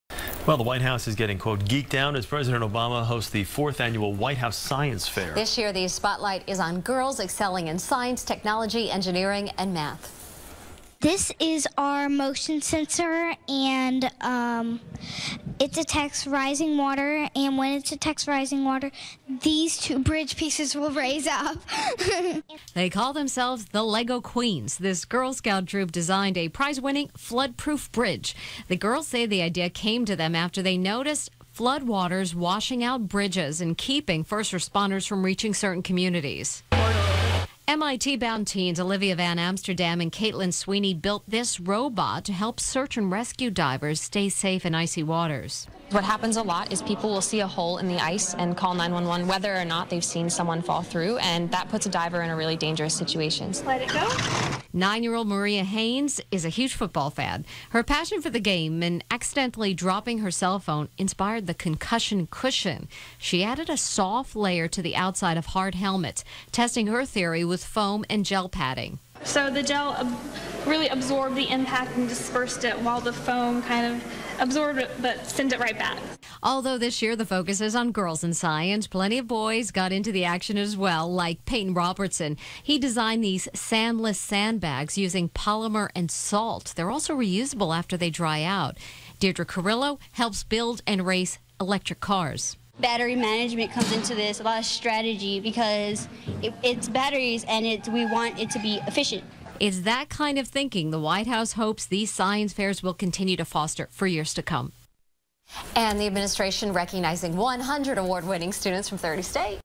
Obama Promotes Math, Tech Aid At Science Fair
President Barack Obama is drawing attention to girls' science and engineering accomplishments at the annual White House Science Fair. CBS 2's Mary Calvi reports.